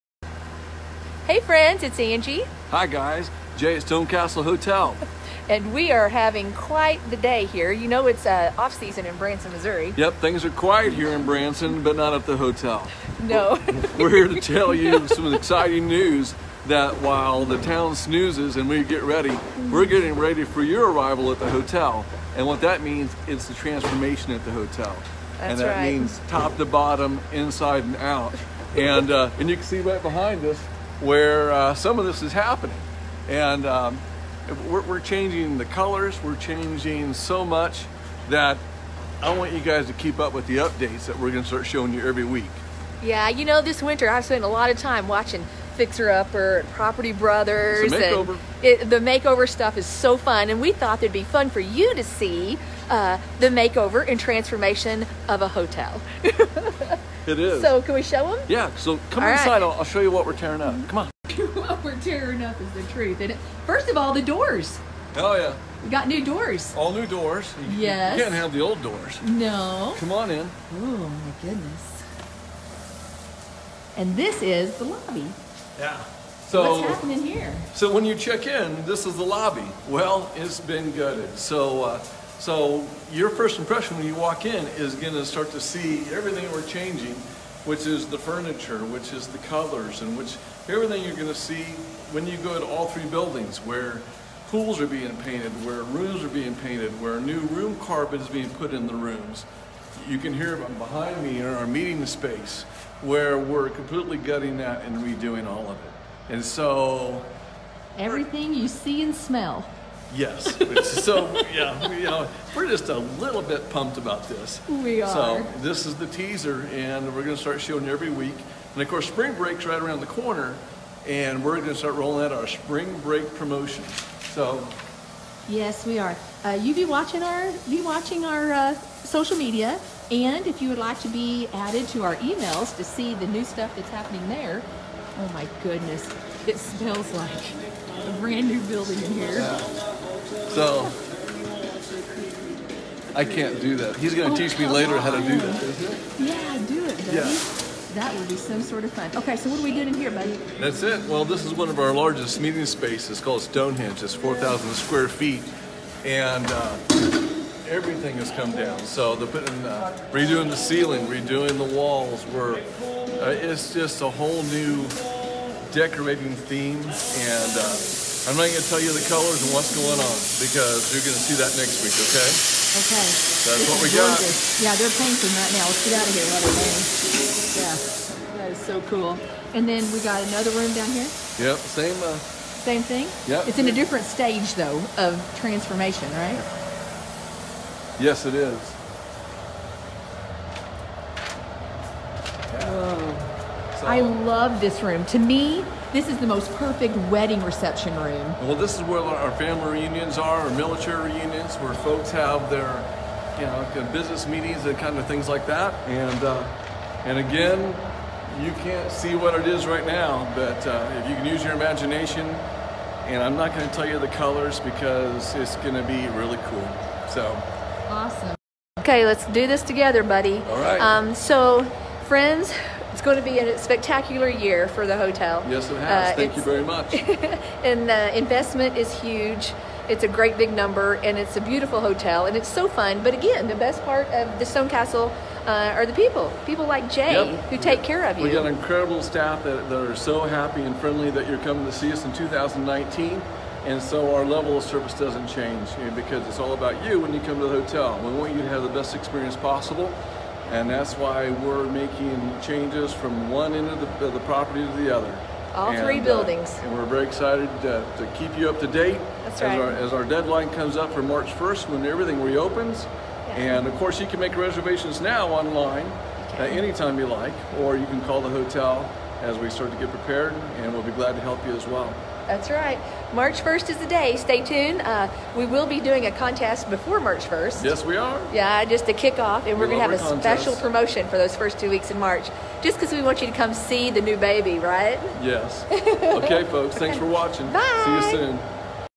Branson Stone Castle Hotel & Conference Center 2019 Transformation
We are making some changes inside and out from the top to the bottom at the StoneCastleHotel&ConferenceCenter. We open on March 1st. Take a peak of the transformation. We will continue to show you more details as opening day gets here! Less than 30 days and you can see the new place!